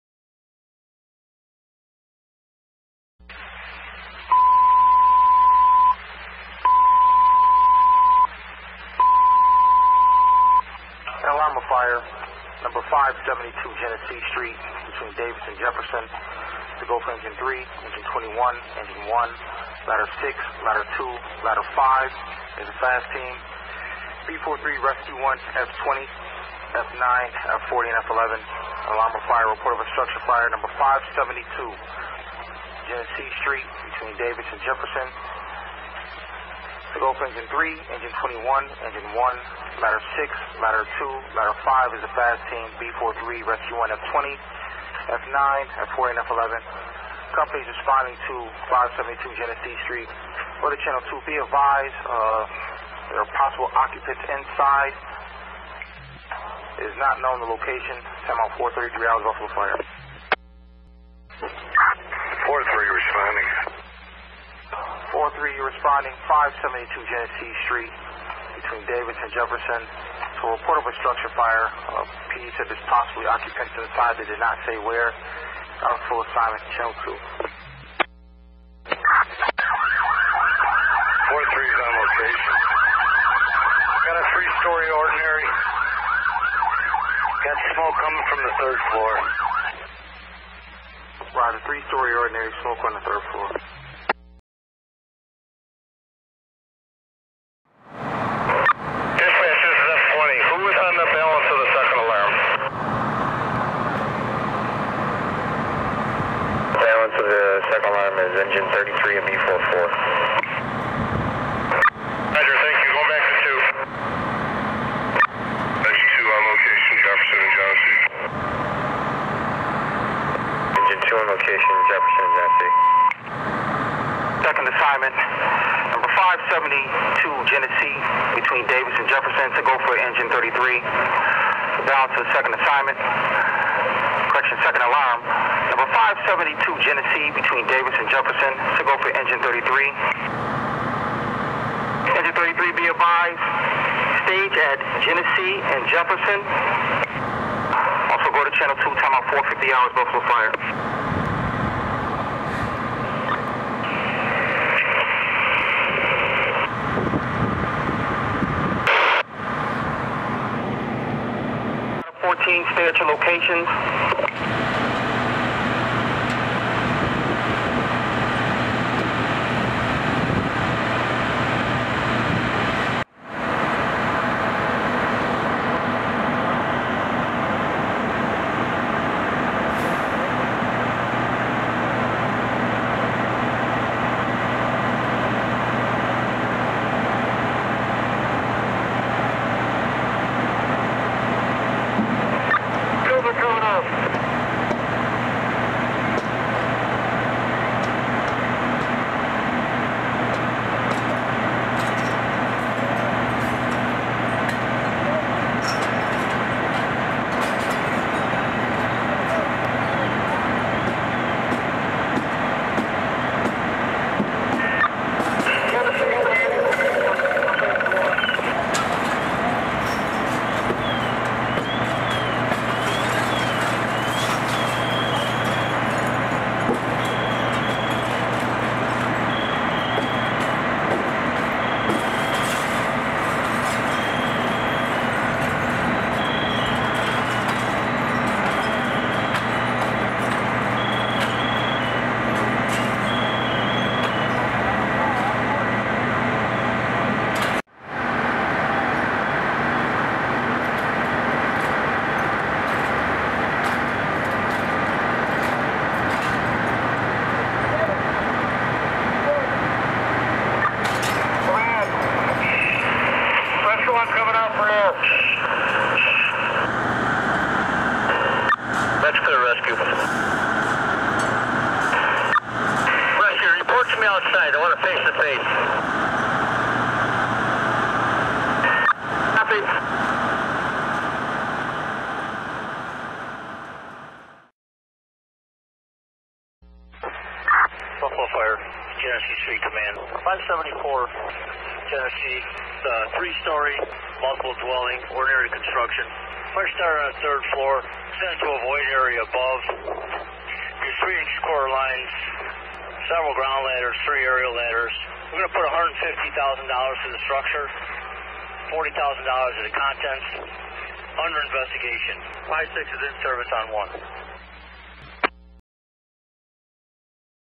Buffalo FD 2nd Alarm - 572 Genesee st
B-43 on scene 3 story ordinary bldg with smoke showing
from 3rd floor. Engine 21 advances lines to 3rd floor. Engine 3 backs up with 2nd line and reports that fire is in the cokcloft. Truck 6 sets up platform to vent flat roof. B-56 requests extra 2 & 2 to scene then balances out 2nd Alarm for manpower. Fire contained to 3rd floor and cockloft above.
3 -1¾" lines in use & 3 Platforms airborne
ON Scene:
Engines: 21, 3, 1, 32, 2, 33  - Trucks: 6, 2, 14, 15, 5 FAST, Rescue 1
Chiefs: B-43, B-44, B-46, B-51 & B-52
Support Staff: F-20, 40, 9, 11, 72 & 76
Damages: $ 190K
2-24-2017   -   Time Out: 04:33 hrs